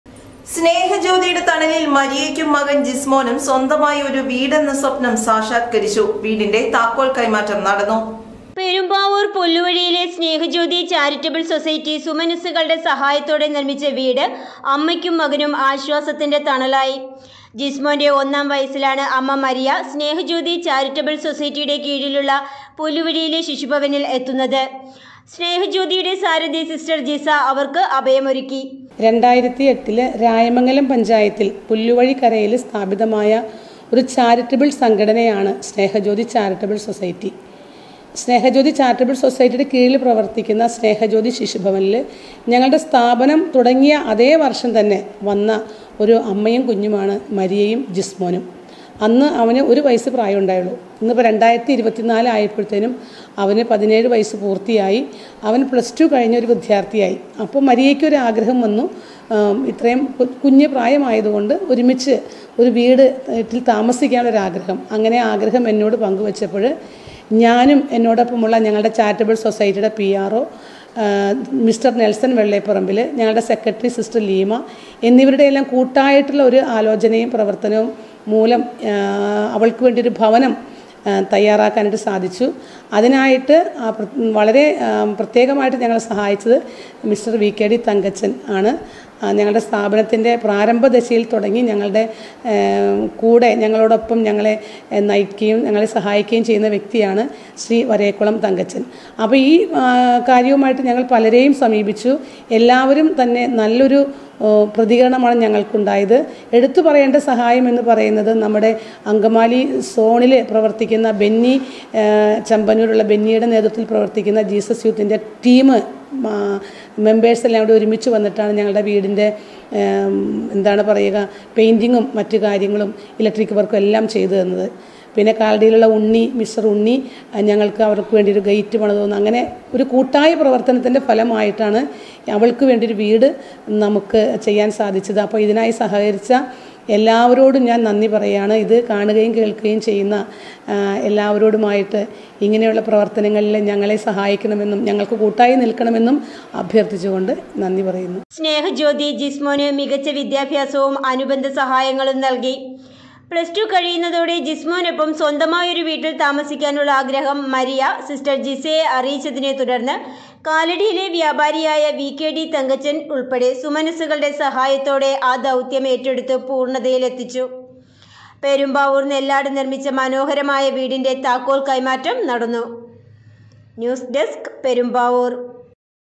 മരിയയ്ക്കും മകൻ ജിസ്‌മോനും സ്വന്തമായി ഒരു വീട് എന്ന സ്വപ്നം സാക്ഷാത്ക്കരിച്ചു.
സ്നേഹജ്യോതി' യുടെ തണലിൽ, മരിയയ്ക്കും മകൻ ജിസ്‌മോനും സ്വന്തമായി ഒരു വീട് എന്ന സ്വപ്നം സാക്ഷാത്ക്കരിച്ചു. വീടിൻ്റെ താക്കോൽ കൈമാറ്റം നടന്നു.